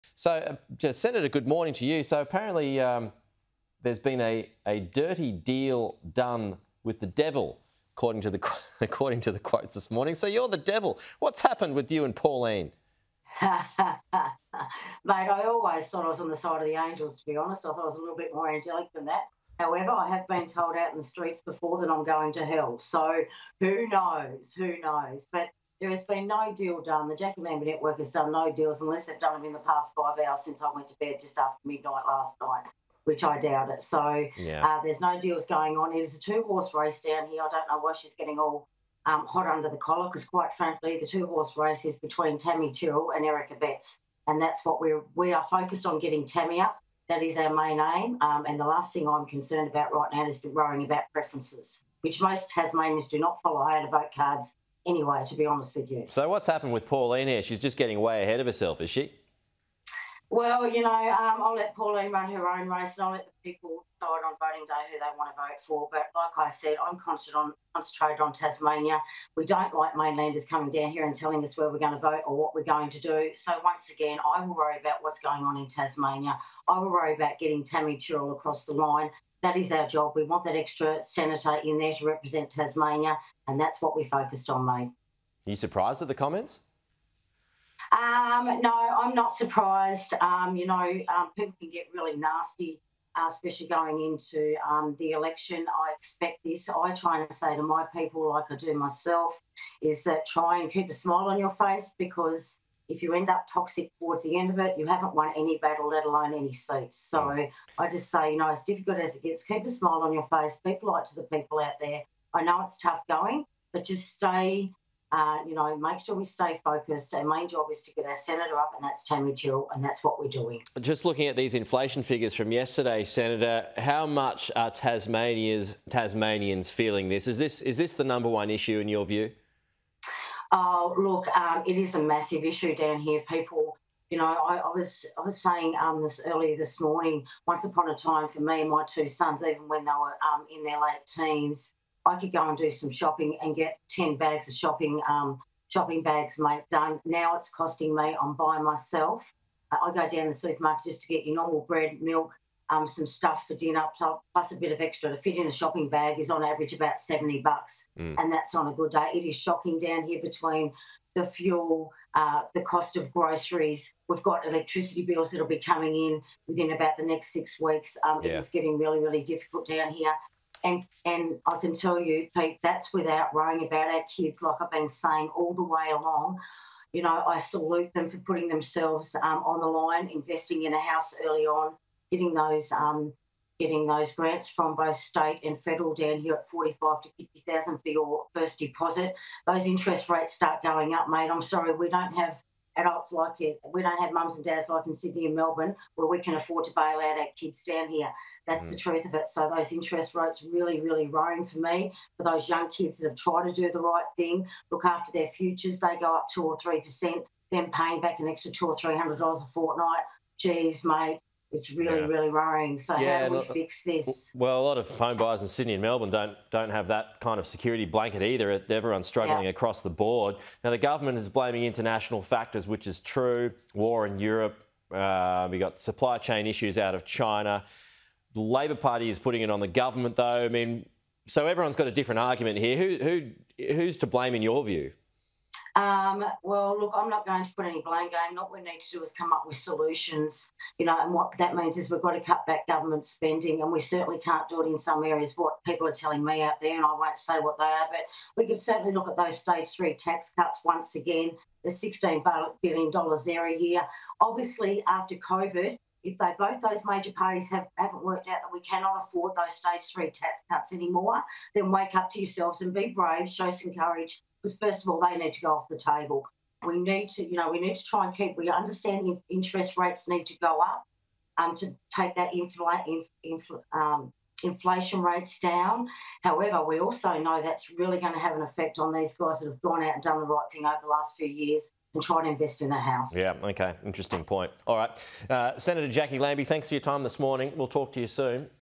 'There's been no deals done': Lambie
Independent Senator Jacqui Lambie says the last thing she is concerned with right now is "worrying about preferences".

"There's been no deal done," Ms Lambie told Sky News Australia.

"There's no deals going on."

"Our main job is to get our Senator up and that's Tammy Tyrrell and that's what we're doing."